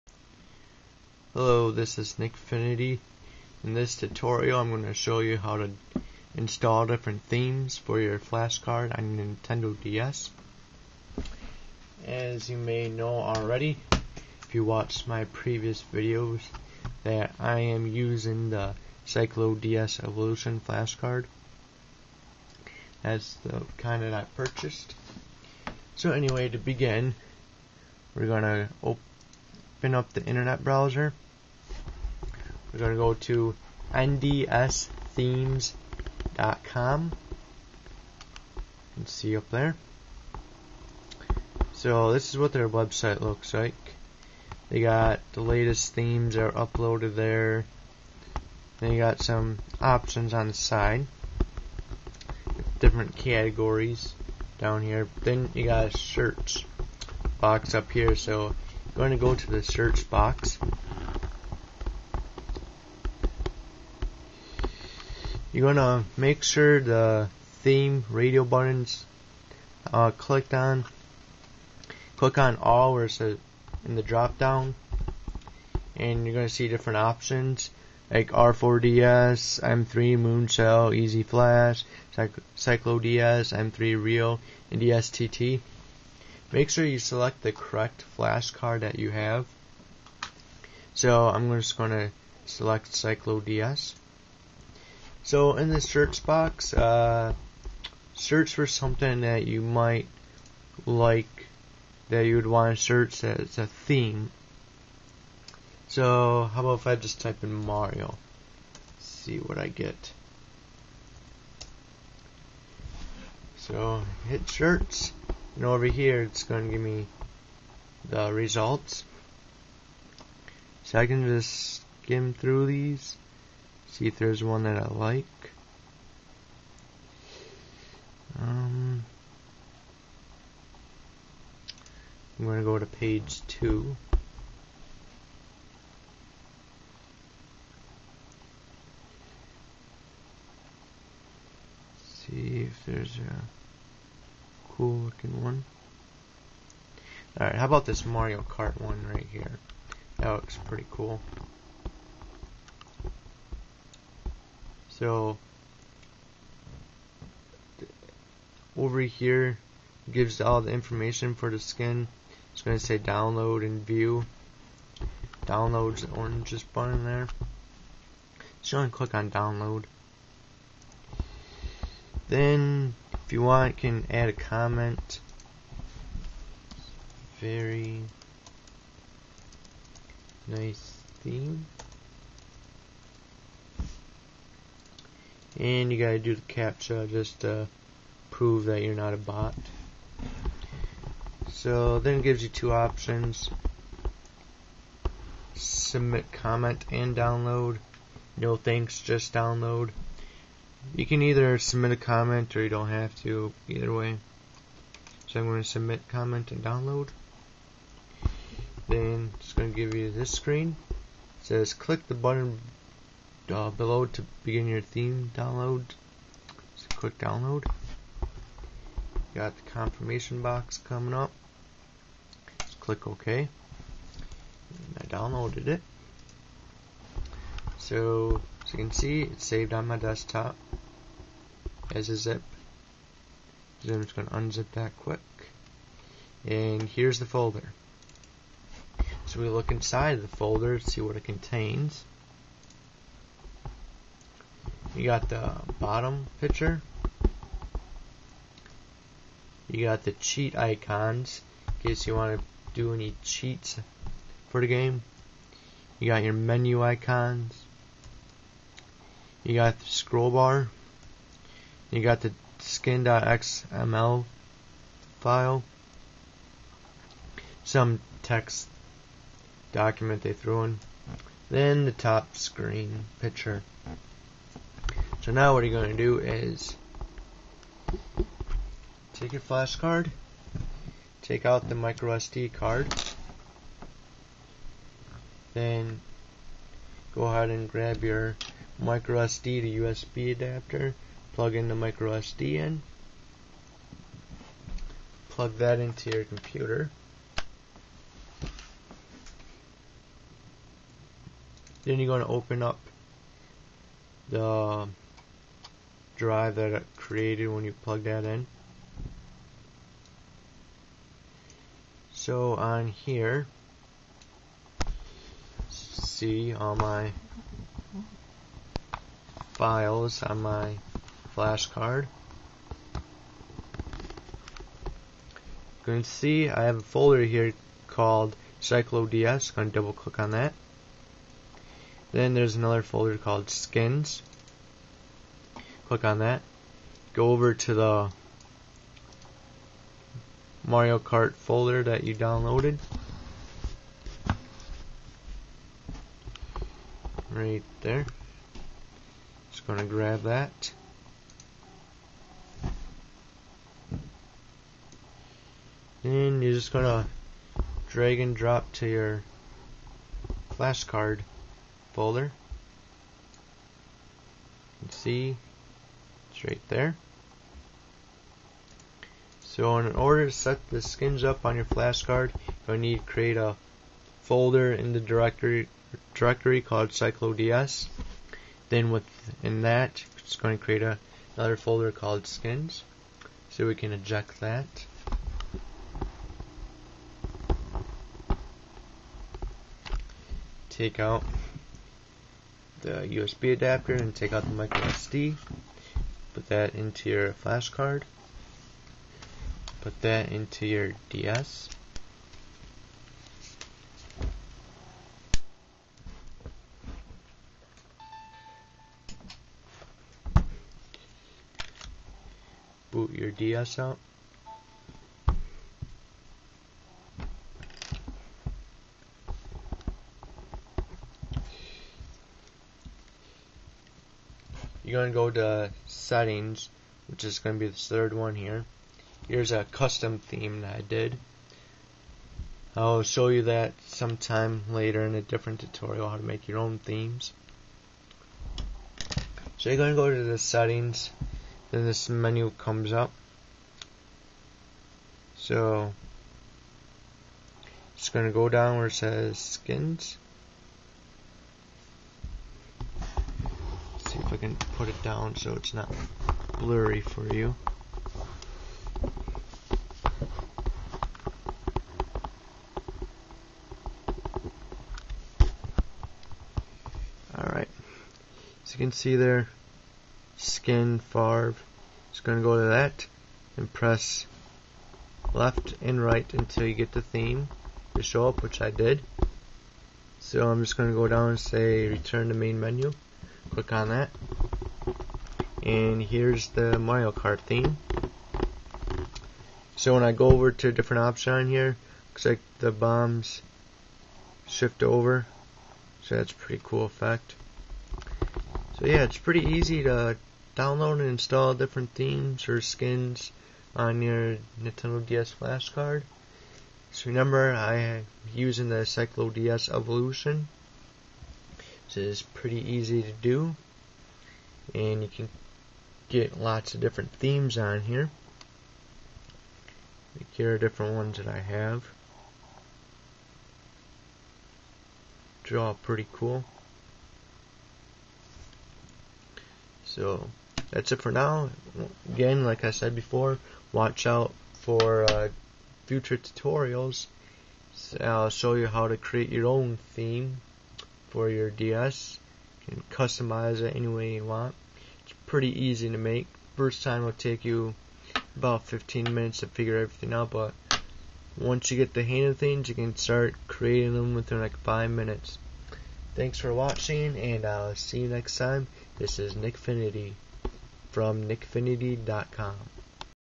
How to install themes on Nintendo DS flash card.
Showing you how to install skins/themes for your flash card on Nintendo DS. The flashcard I use is the CycloDS Evolution.